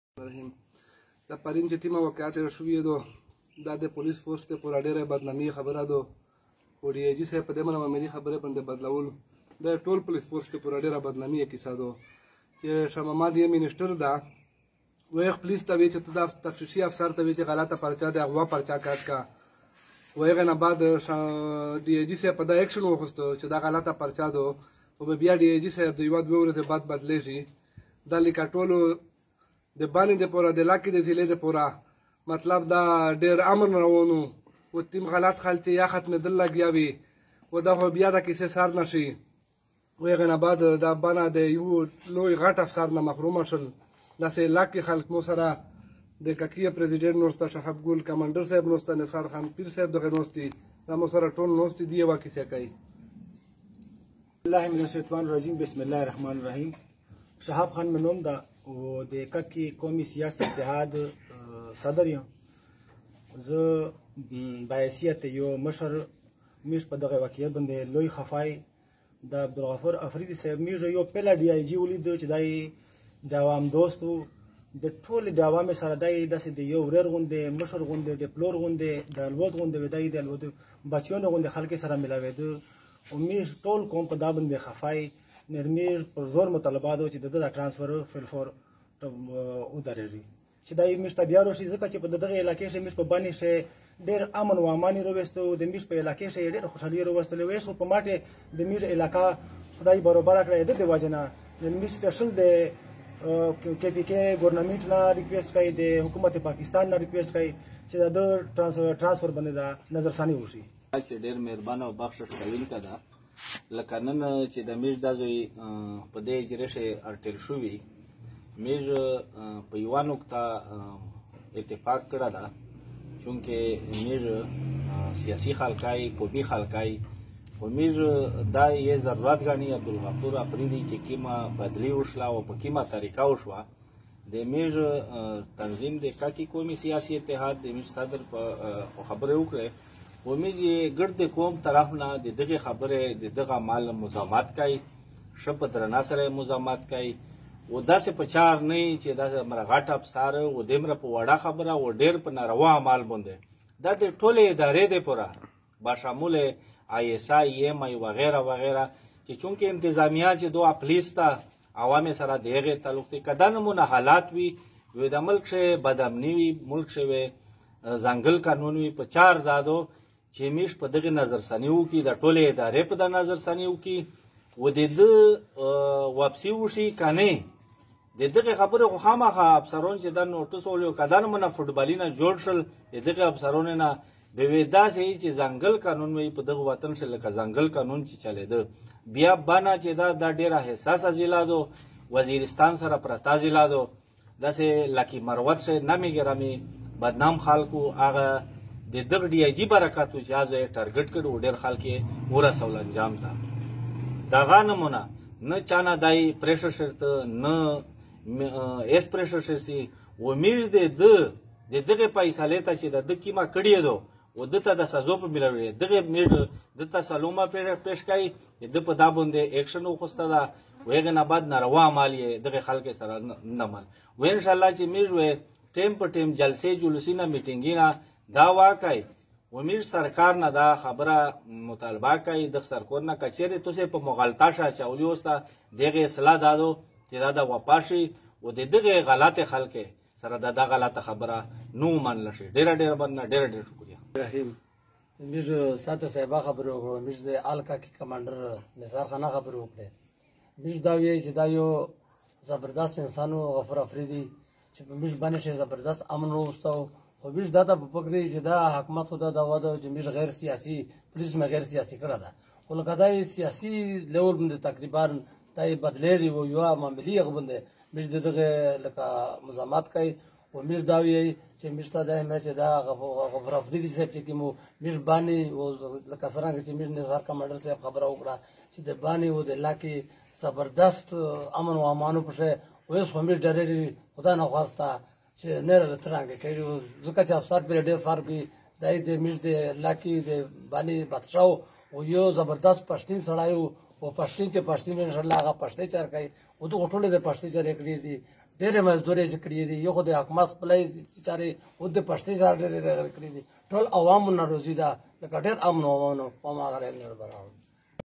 * Drug and trafficking mafia overwhelmed * DIG Bannu Abdul Ghafoor Afridi has been transferred by Pr
* Drug and trafficking mafia overwhelmed *
DIG Bannu Abdul Ghafoor Afridi has been transferred by Provincial Minister Shah Mohammad Wazir. The accident took place yesterday between a police constable and the car of Shah Mohammad Wazir's son. After the altercation, Shah Mohammad Wazir pressured the investigating officer to include kidnapping provisions in the FIR, but police officials refused to register a false FIR by bringing it to the notice of DIG Bannu Abdul Ghafoor Afridi. Shah Mohammad Wazir complained to the Chief Minister. The IG immediately transferred DIG Bannu Abdul Ghafoor Afridi to please the provincial minister and directed him to report to the Central Police Office.